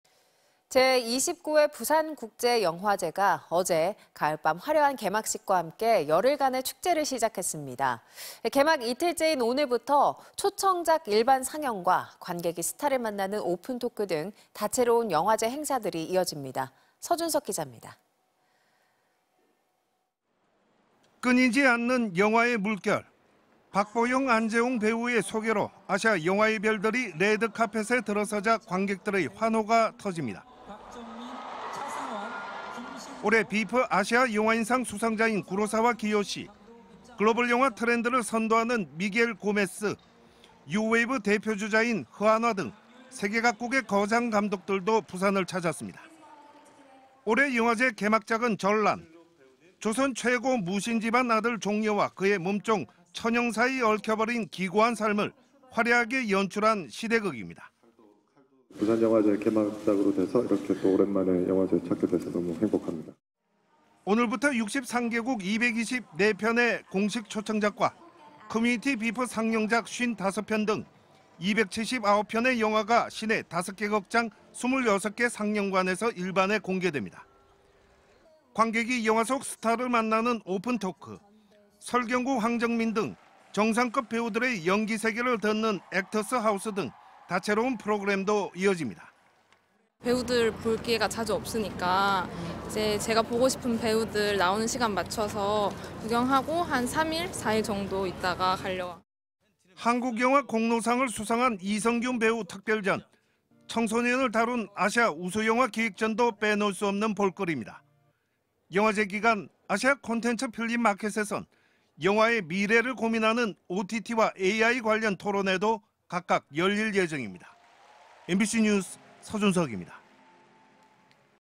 부산국제영화제 개막‥초청작 일반 상영 시작 (2024.10.03/뉴스투데이/MBC)
제29회 부산국제영화제가 어제 가을밤 화려한 개막식과 함께 열흘간의 축제를 시작했습니다.

개막 이틀째인 오늘부터 초청작 일반 상영과 관객이 스타를 만나는 오픈 토크 등, 다채로운 영화제 행사들이 이어집니다.